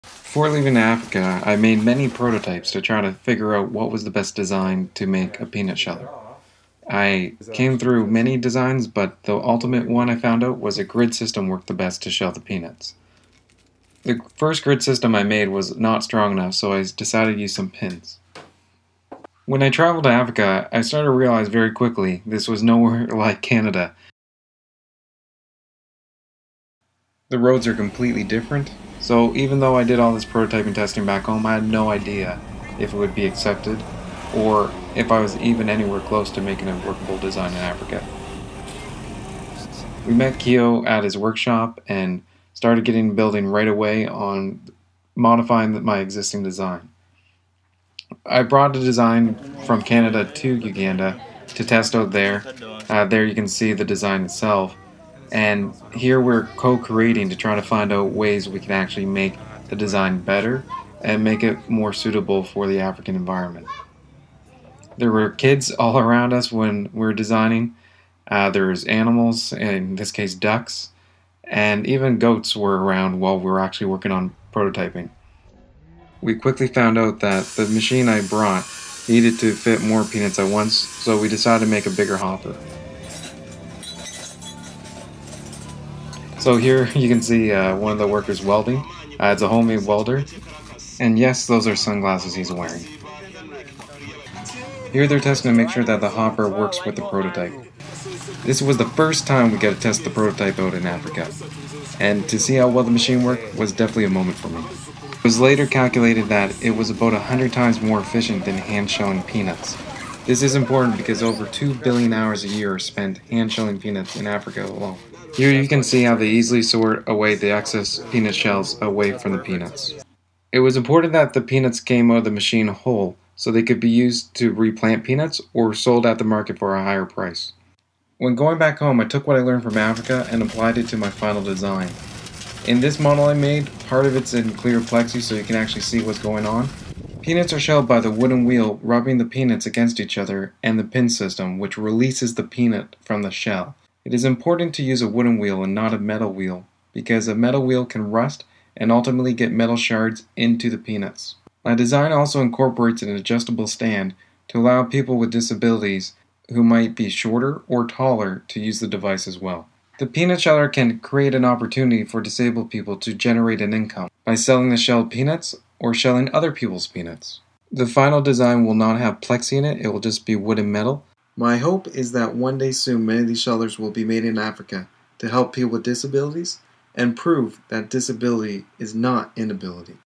Revolutionary Hand Powered African Peanut Sheller
**Update** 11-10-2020 This is a project I have hoped to for a long time to optimize a final design and release free plans on how to make one on your own. It originated as a school project where the demand for it grew more than I thought.  I still hope to get back to finalizing a design, and working the few issues the current design has. I know in the past I have given different estimates on time when I planned on releasing the final design, but at this current time I don't know when that will be. For the mean time please use this video for inspiration.

2 billion hours a year are spent a year spent hand shelling peanuts. The African Peanut Sheller is 100x faster then hand shelling and would save 1.98 billion hours. That is equivalent to 226 027 years every year!
The African Peanut Sheller is designed to be manufactured locally in Africa for people with disabilities. Materials used to make the sheller can also be sourced locally. Templates make it extremely easy to make the parts without even needing a ruler. The pin system allows a complex, circular shape to be made by simply drilling holes. The sheller can be made at a low cost and is hand powered.
The Sheller incorporates an adjustable stand to to help accommodate height variances for people with disabilities. By travelling to Kasese, Uganda it was discovered that people often travel to people businesses that have machines like a sheller. People with mobile disabilities who own a Sheller would be able to generate income with the device by having people come to shell their peanuts. The African Peanut sheller also has an extremely low breakage rate. This is important because damaged peanuts cannot be used to plant more crops and whole peanuts sell for more money on the market compared to broken peanuts.